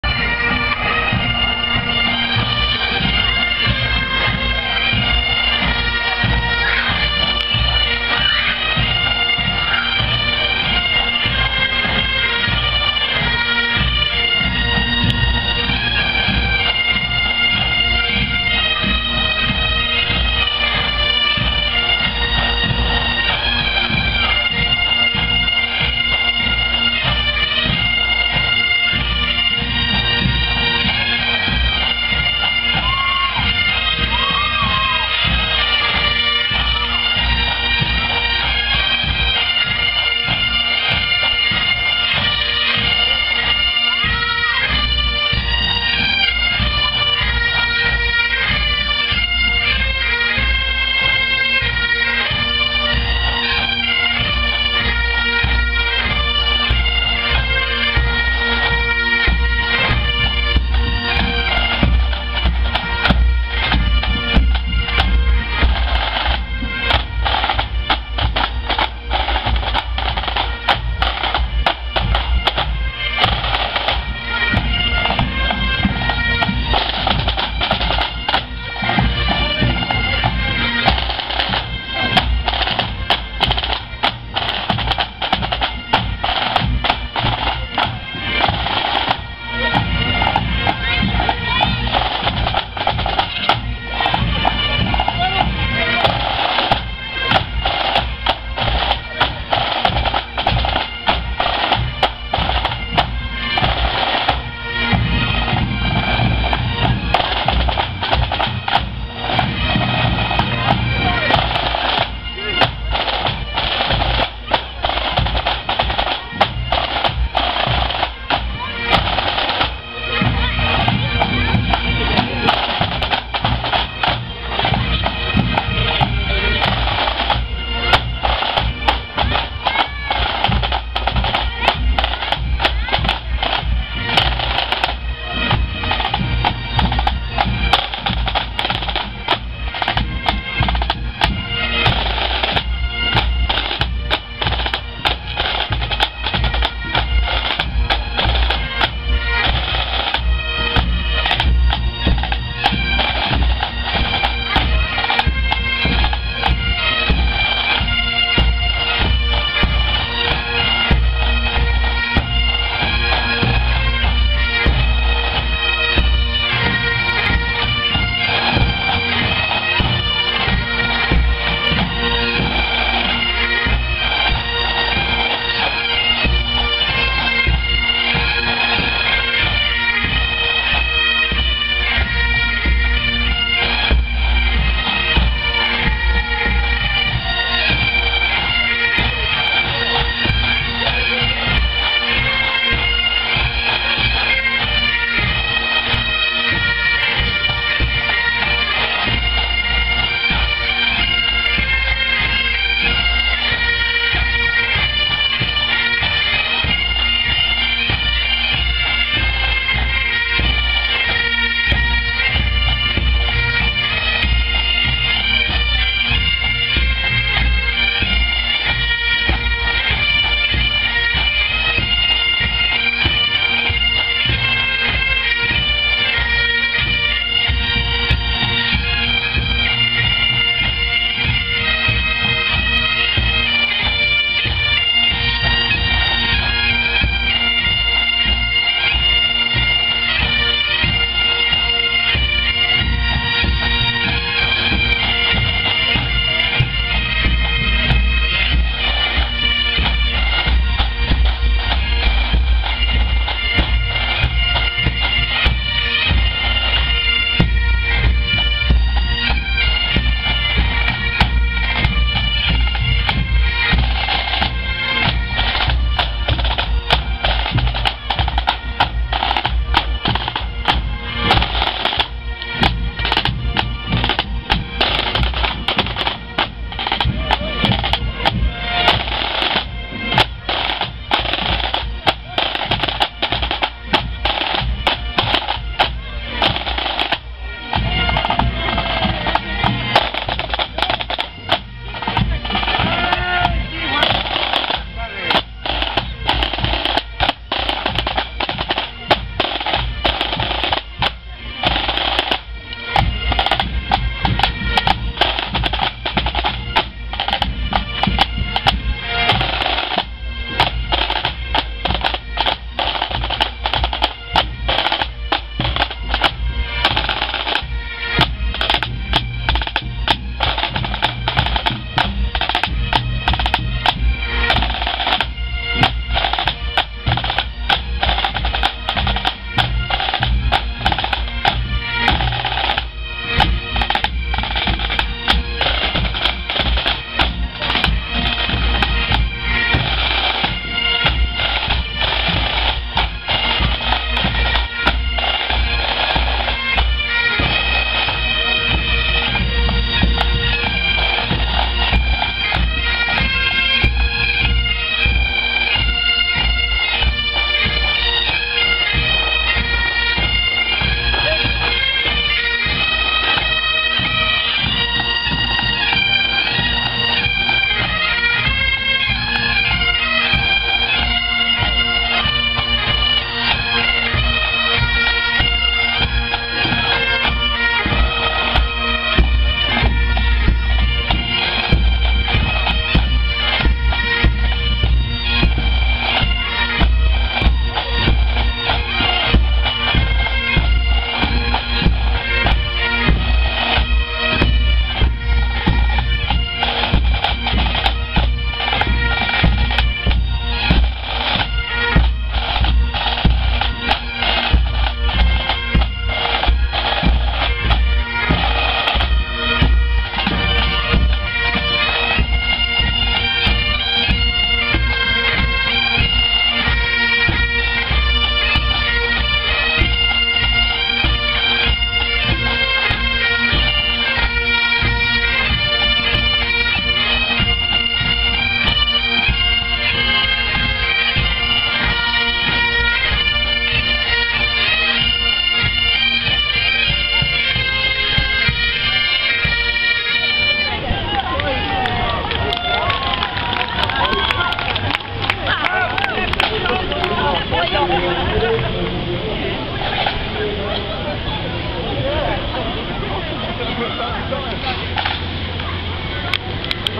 Massed Pipes and Drums at Braemar Night in Blairgowrie 2015
Me join Coupar Angus Burgh, Alyth, Forfar and Blairgowrie and Rattray pipe bands for there Massed Pipes and Drums match from British Legion club to Wellmeadow for Braemar Night in Blairgowrie on 5th September 2015

Video by my dad